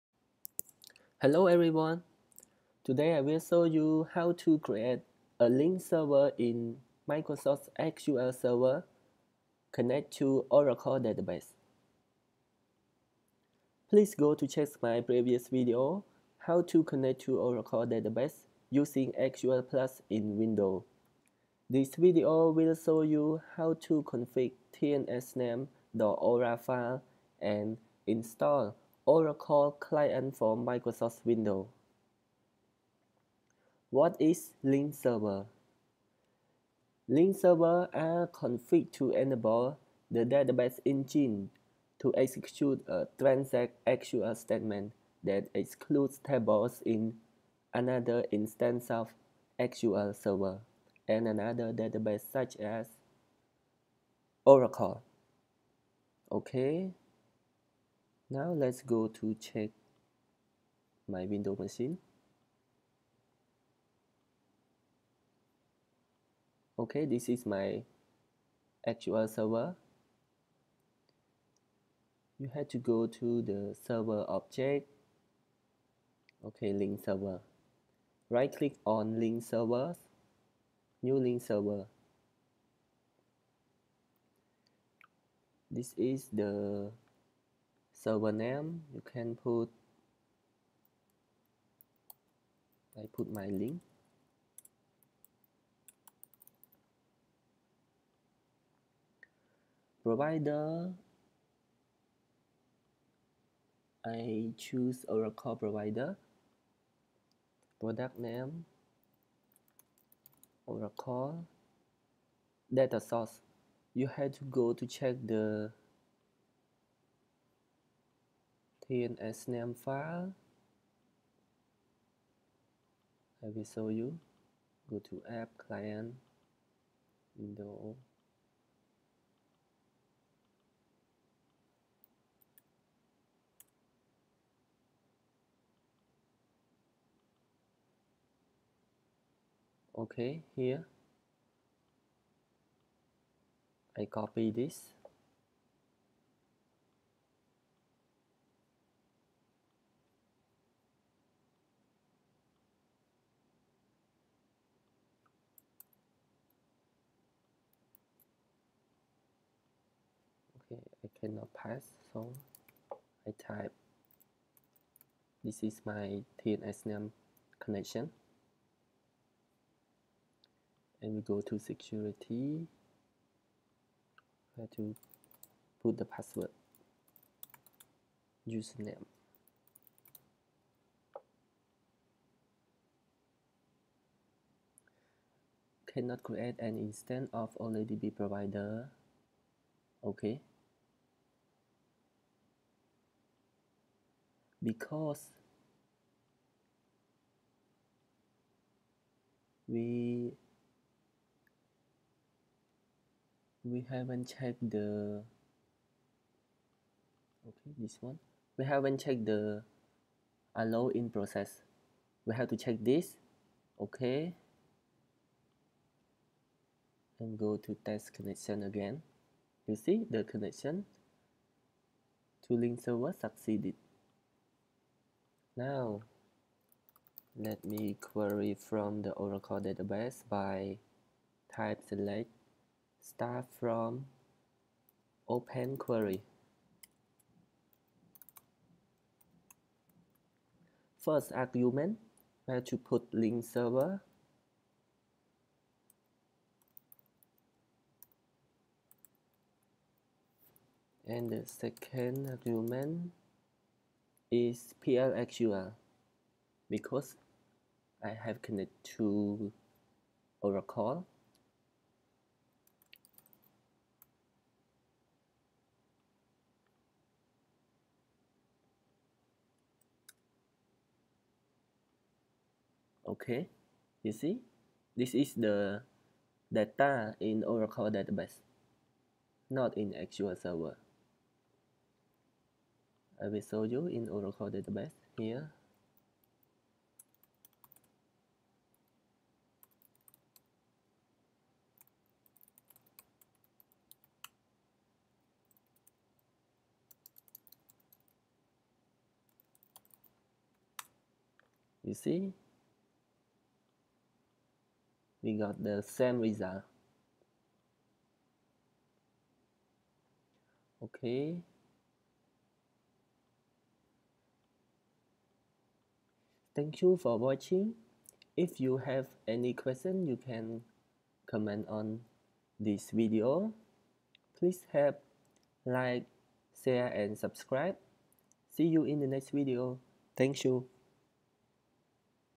Sql server dba - how to configure linked server to connect to oracle database
In this tutorial "Sql server dba - how to configure linked server to connect to oracle database" will show you step by step how to create linked server in Microsoft Sql Server 2016 and select data from oracle 12c database.

Thanks,

Sql server dba
Microsoft Sql Server 2016
Microsoft Sql Server Linked Server